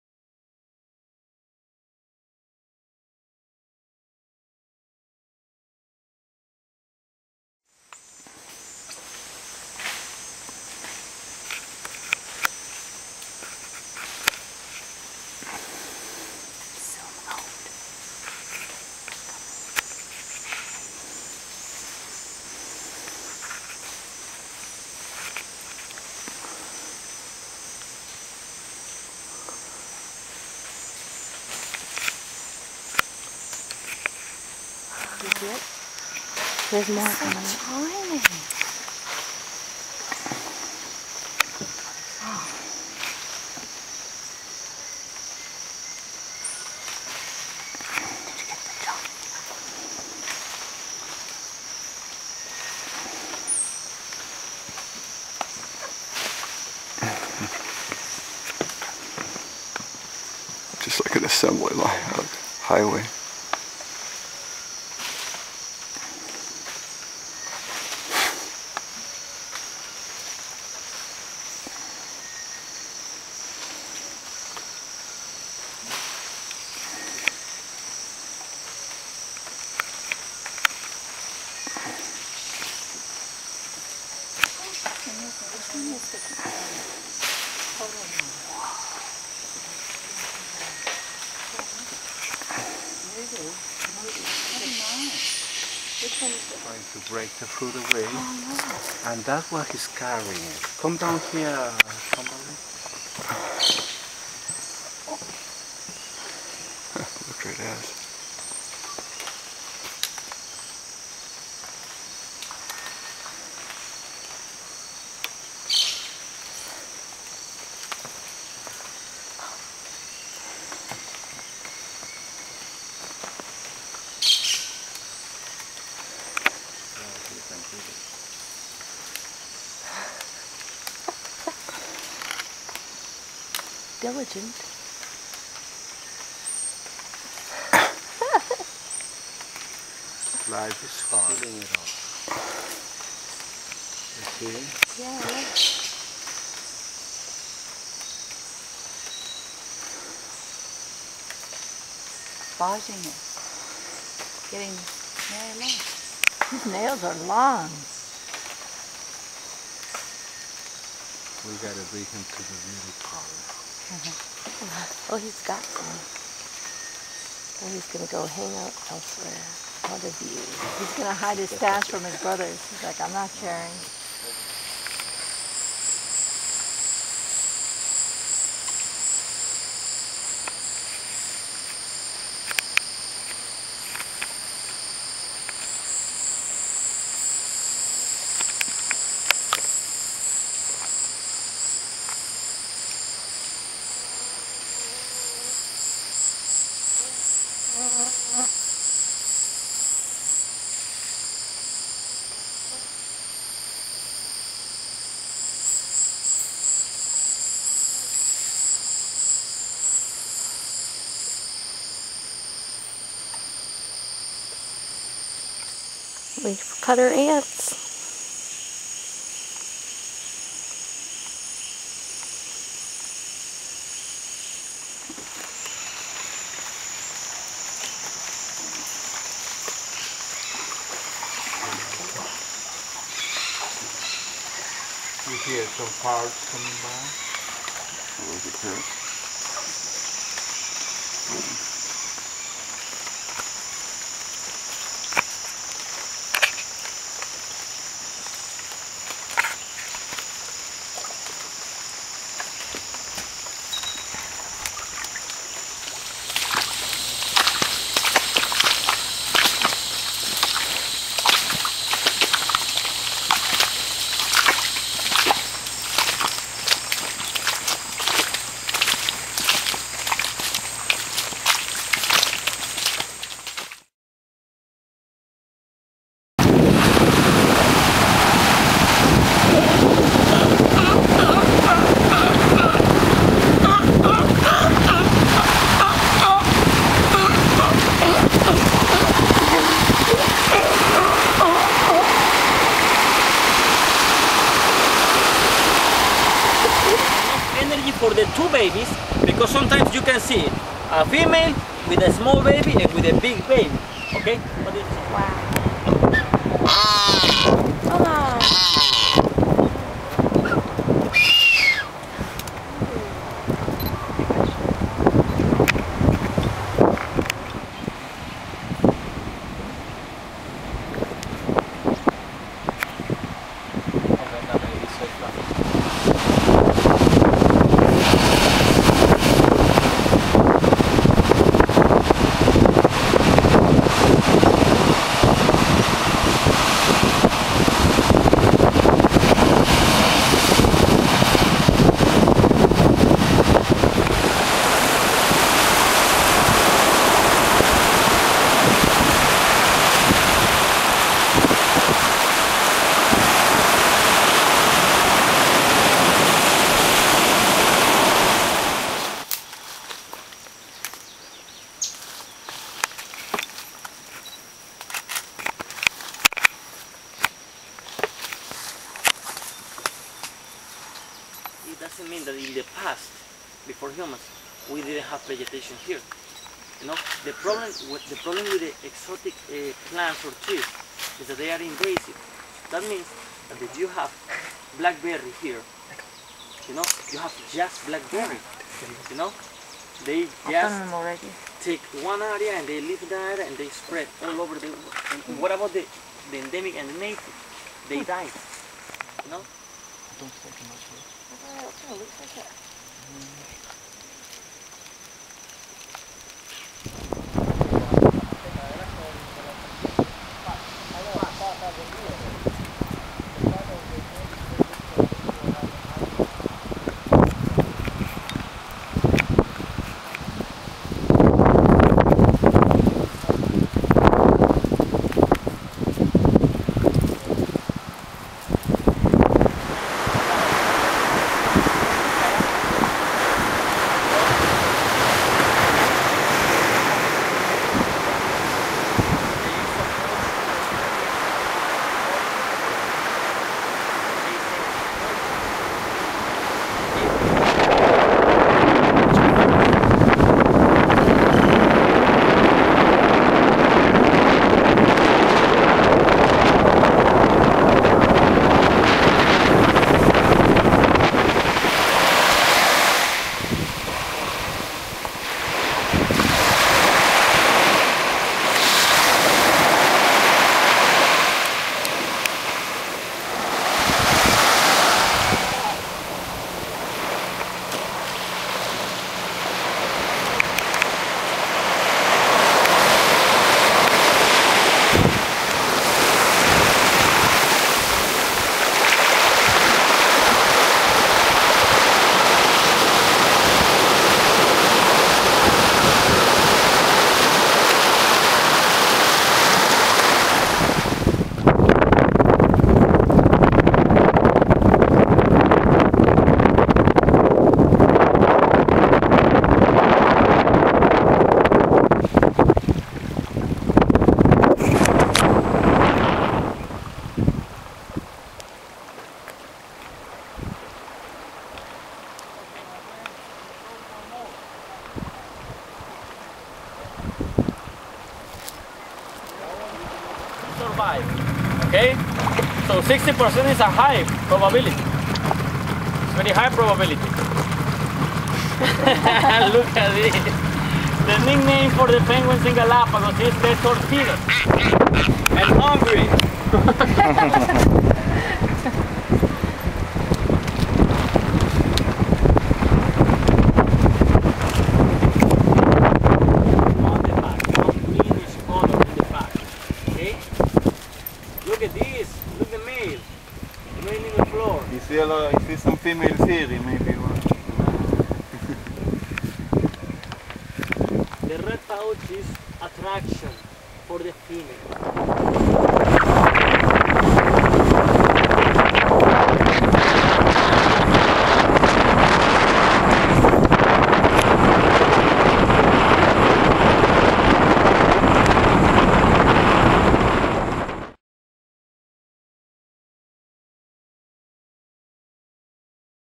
Jon and Joans Excellent Adventure in Ecuador
Video highlights of wildlife and experiences from our trip to the Amazon and the Galápagos Islands. Wildlife includes squirrel monkeys, white-faced capuchin monkey, pygmy marmoset, leaf cutter ants, Galápagos sea lions, Blue-footed boobies, Waved albatross and baby, Giant Tortoise, a Darwins finch, Sally lightfoot crab, Galápagos land iguana, Oystercatcher, Red-billed tropicbird, Brown pelican, Galápagos penguin, Baby boobie, Great frigatebird.